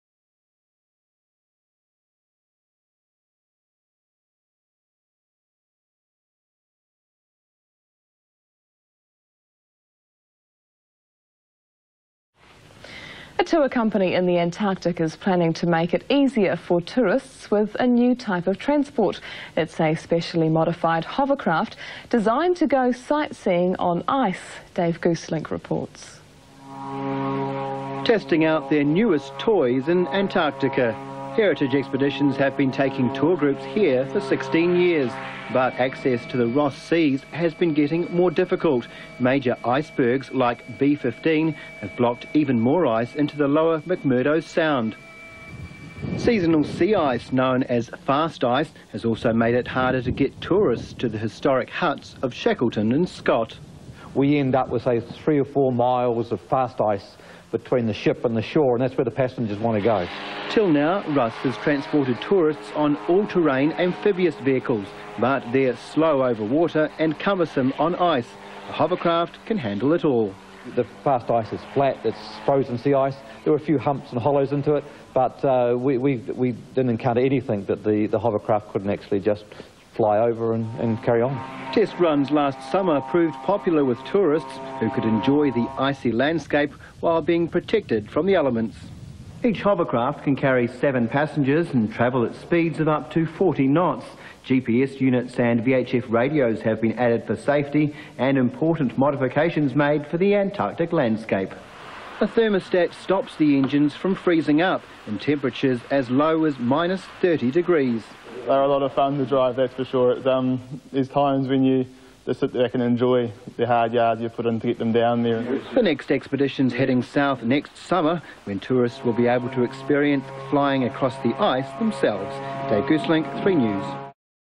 Экотуризм на судах на воздушной подушке (СВП) / Ecotourism and hovercrafts (часть 1)
Использование судов на воздушной подушке в экотуризме на примере СВП австралийской компании Airlift Hovercraft (часть 1).